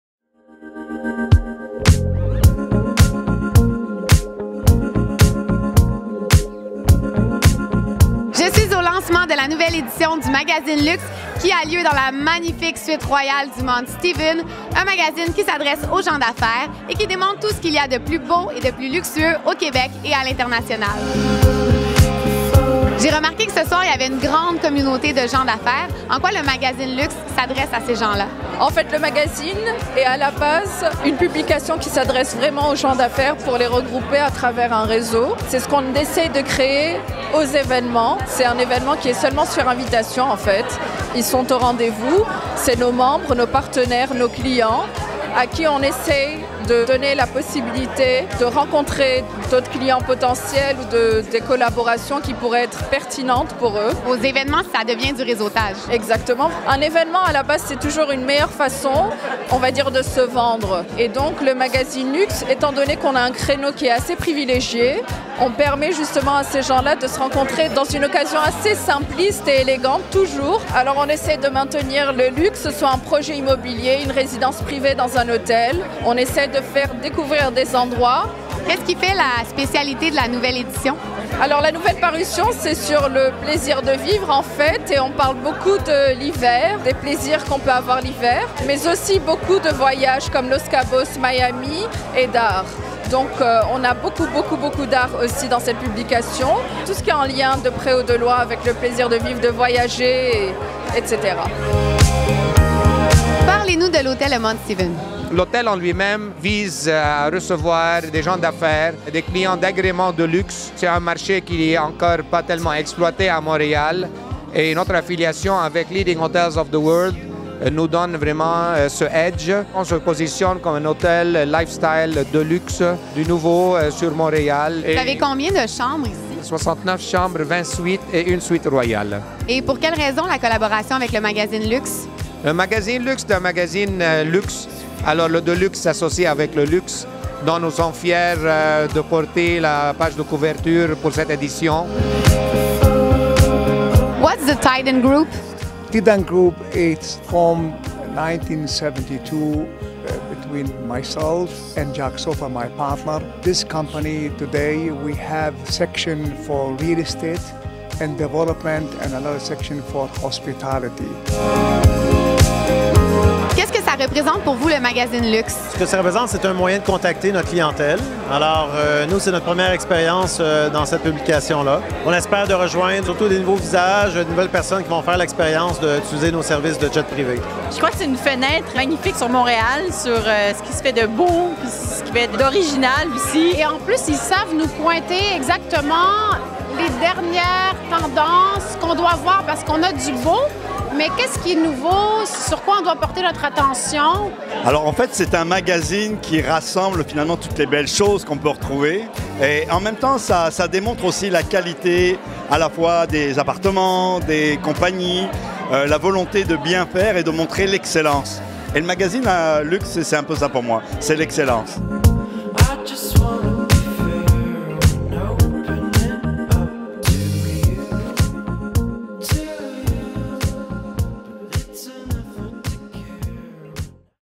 Lancement du Magazine LUXE SAMIA EL à Montréal | Montreal.TV
C'est dans la magnigique Suite royale de l'Hôtel Mount Stephen que le dernier numéro du Magazine Luxe a été lancé.  La soirée sur invitation seulement a permis aux convives de découvrir le magazine dans une ambiance conviviale et festive.  Dans ce reportage, la présidente de Luxe Magazine, Samia El, les propriétaires du Mount Stephen et fondateurs du Groupe Tidan, Jack Sofer et Mike Yuval, le directeur général du Mount Stephen, Antoine Naoum, le président de Vinci Aviation, Stéphane Prud'Homme, la fondatrice du Balnea Spa, Stéphanie Émond, la fondatrice de Stylissme Communications, Nadine Lajoie et le chroniqueur gastronomie, Thierry Daraize.

Reporter: Audrey Gélinas.